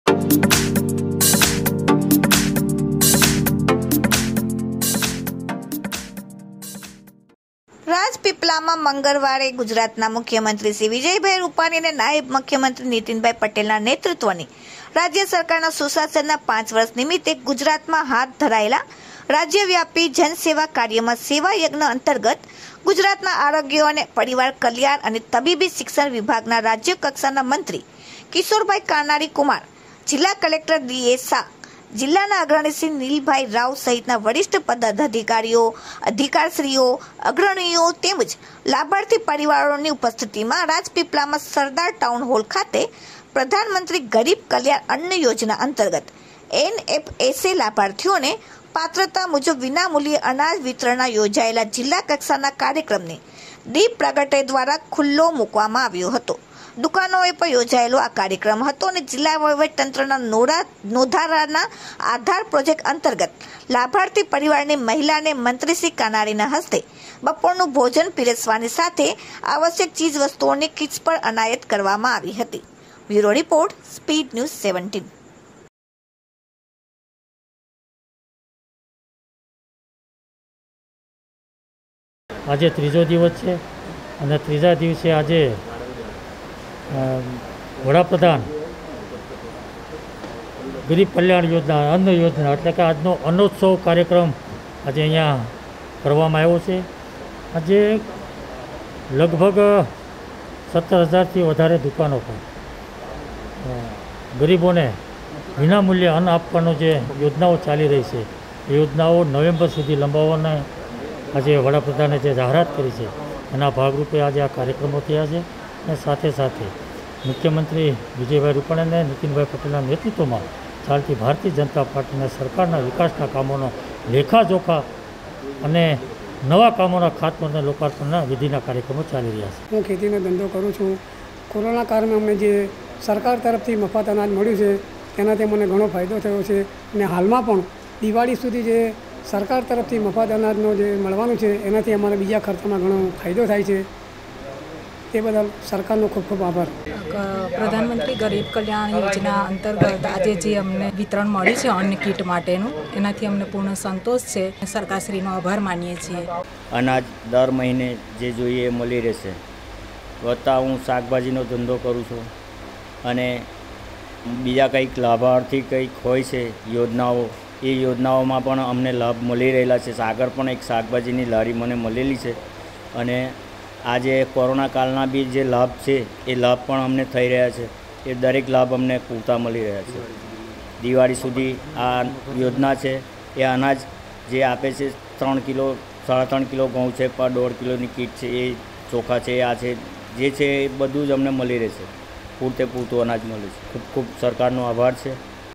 NARMBDA રાજય સરકારના સુસાશનના પાંચ વર્ષ નિમિત્તે સરકારે પ્રજાકલ્યાણની અનેકવિધ યોજનાઓ અને વિકાસકામ
રાજય સરકારના સુસાશનના પાંચ વર્ષ નિમિત્તે સરકારે પ્રજાકલ્યાણની અનેકવિધ યોજનાઓ અને વિકાસકામો થકી
જનસુખાકારીમાં વૃધ્ધિ થાય તે દિશામાં કરેલી કામગીરીના લેખાજોખાં જનજન સુધી પહોંચાડવાનો સેવાયજ્ઞ આદર્યો છે

- આરોગ્ય રાજય મંત્રીશ્રી કિશોરભાઇ કાનાણી (કુમાર)
પ્રધાનમંત્રી ગરીબ કલ્યાણ અન્ન યોજના અંતર્ગત નર્મદા જિલ્લામાં કુલ-૯૯,૫૧૪ પરિવાર-કુટુંબોની
૪,૯૦,૭૮૮ જેટલી જન સંખ્યાને નવેમ્બર,૨૦૨૧ સુધી યોજનાનો મળનારો લાભ
રાજપીપલામાં મંત્રીશ્રીની ઉપસ્થિતિમાં પ્રધાનમંત્રી ગરીબ કલ્યાણ અન્ન યોજના અંતર્ગત “NFSA” લાભાર્થીઓને
પાત્રતા મુજબ વિના મૂલ્યે અનાજ વિતરણનો જિલ્લાકક્ષાનો યોજાયેલો કાર્યક્રમ : જિલ્લાની ૨૨૧ વાજબી ભાવની

દુકાનોએ પણ યોજાયેલા કાર્યક્રમ

જિલ્લા વહિવટીતંત્રના “નોંધારાના આધાર” પ્રોજેક્ટ અંતર્ગત લાભાર્થી પરિવારની મહિલાને મંત્રીશ્રી કાનાણીના હસ્તે

બપોરનું ભોજન પીરસવાની સાથે આવશ્યક ચીજ વસ્તુઓની કિટ્સ એનાયત

રાજપીપલા,મંગળવાર :- ગુજરાતના મુખ્યમંત્રીશ્રી વિજયભાઇ રૂપાણી અને નાયબ મુખ્યમંત્રીશ્રી
નિતિનભાઇ પટેલના નેતૃત્વની રાજય સરકારના સુસાશનના પાંચ વર્ષ નિમિત્તે ગુજરાતમાં હાથ ધરાયેલા
રાજયવ્યાપી જનસેવા કાર્યના સેવા યજ્ઞ અંતર્ગત ગુજરાતના આરોગ્ય અને પરિવાર કલ્યાણ અને તબીબી
શિક્ષણ વિભાગના રાજયકક્ષાના મંત્રીશ્રી કિશોરભાઇ કાનાણી (કુમાર), જિલ્લા કલેકટરશ્રી ડી.એ.શાહ,
જિલ્લાના અગ્રણીશ્રી નીલભાઇ રાવ સહિતના વરિષ્ઠ પદાધિકારીશ્રીઓ, અધિકારીશ્રીઓ, અગ્રણીઓ તેમજ
લાભાર્થી પરિવારોની ઉપસ્થિતિમાં રાજપીપલામાં સરદાર ટાઉન હોલ ખાતે પ્રધાનમંત્રી ગરીબ કલ્યાણ અન્ન
યોજના અંતર્ગત “NFSA” લાભાર્થીઓને પાત્રતા મુજબ વિના મૂલ્યે અનાજ વિતરણના યોજાયેલા
જિલ્લાકક્ષાના કાર્યક્રમને દિપ પ્રાગટય દ્રારા ખૂલ્લો મૂકાયો હતો.